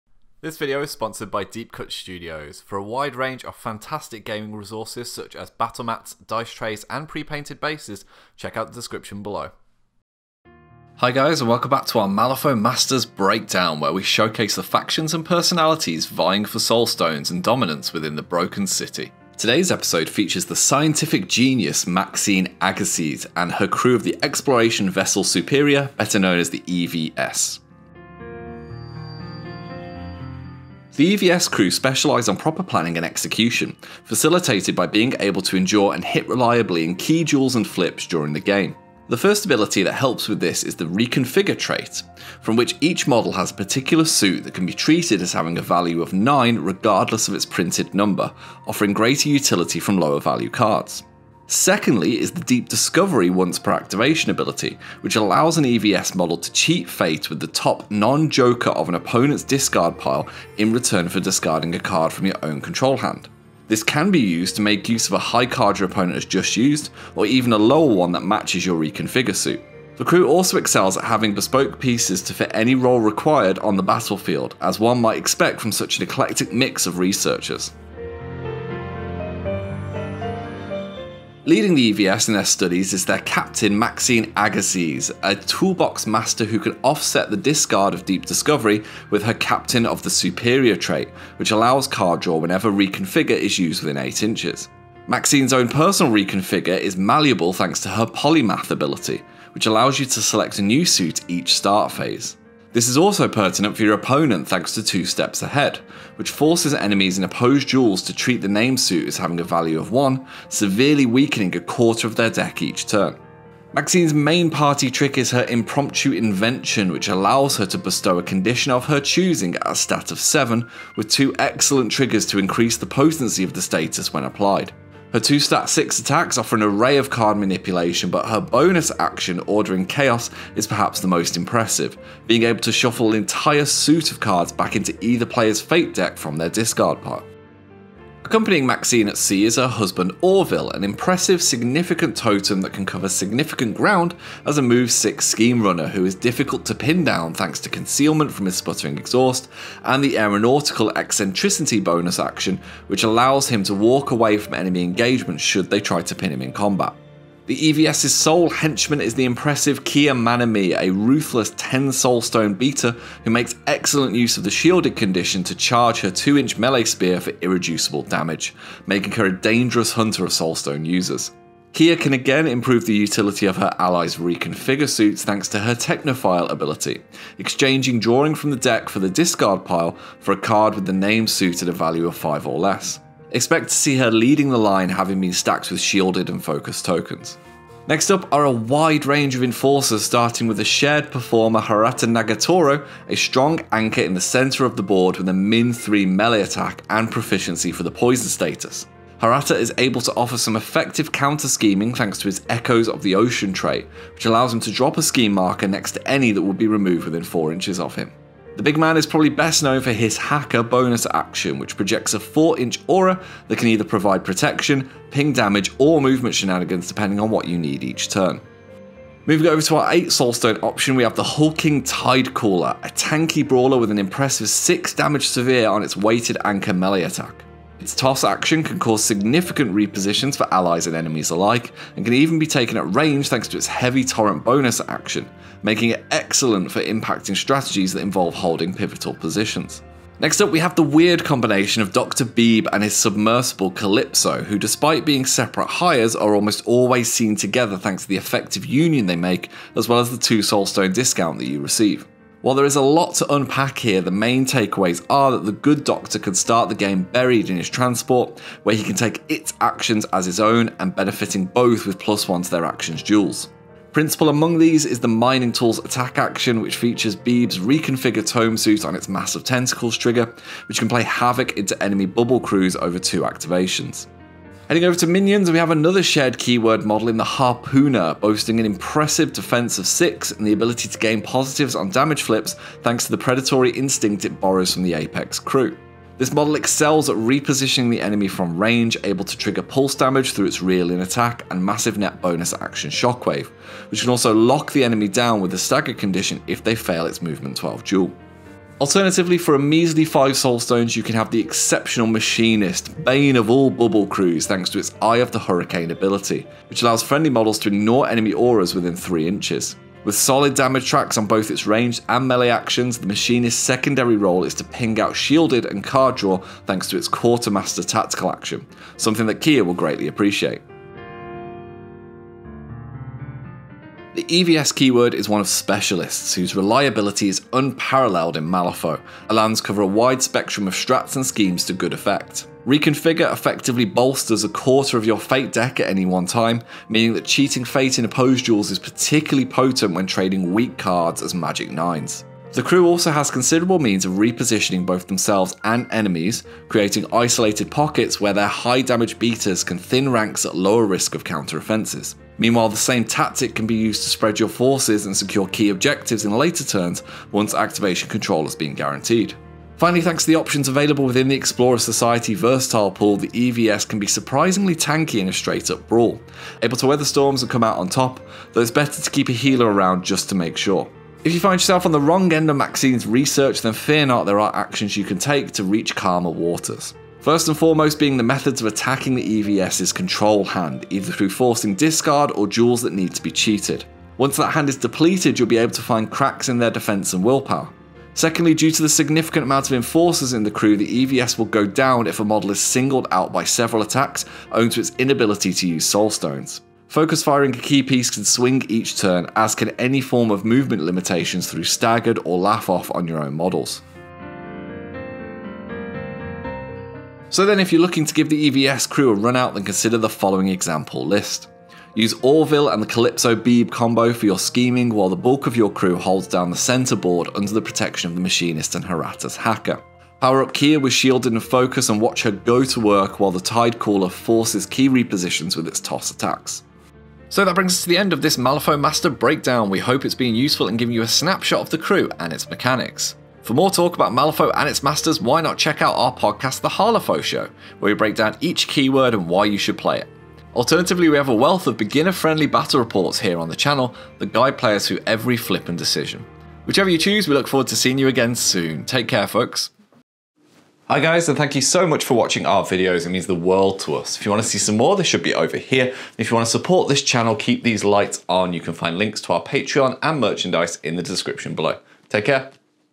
Malifaux Masters: Maxine Agassiz [Explorer's Society]
Tom talks you through the polymath researcher Maxine Agassiz and what her EVS keyword brings to the table!!